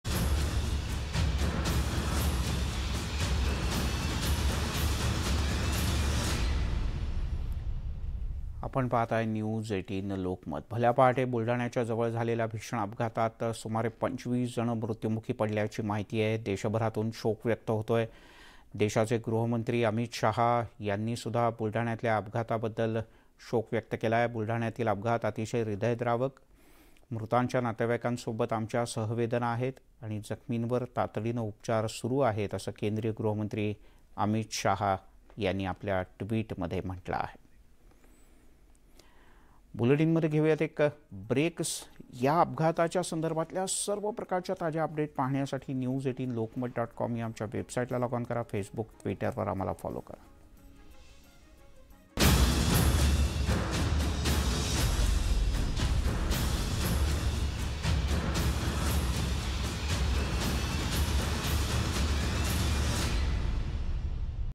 Amit Shah on Buldhana Bus Accident | अमित शाहांचं बुलढाणा अपघातावर ट्विट | Marathi News
A terrible accident of a passenger bus took place in Buldhana. 26 passengers have died on the spot. There were a total of 33 passengers in the bus. Eight of these passengers got out safely. The accident took place on Samrudhi Highway near Pimpalkhuta village near Sindkhedaraja in Buldhana district. This passenger bus was going from Nagpur to Pune. At that time, this terrible incident took place around half past midnight.

बुलढाण्यात (Buldhana) एका प्रवासी बसचा भीषण अपघात (Accident) झाल्याची घटना घडली. यामध्ये 26 प्रवाशांचा जागीच मृत्यू झाला आहे. बसमध्ये एकूण 33 प्रवासी होते. यातील आठ प्रवासी सुखरुप बाहेर पडले आहेत. बुलढाणा जिल्ह्यातील सिंदखेडराजाजवळ पिंपळखुटा गावालगत समृध्दी महामार्गावर हा अपघात झाला आहे. ही प्रवासी बस नागपूरहून पुण्याला निघाली होती. त्यावेळी रात्री दीड वाजण्याच्या सुमारास ही भीषण घटना घडली.
n18oc_news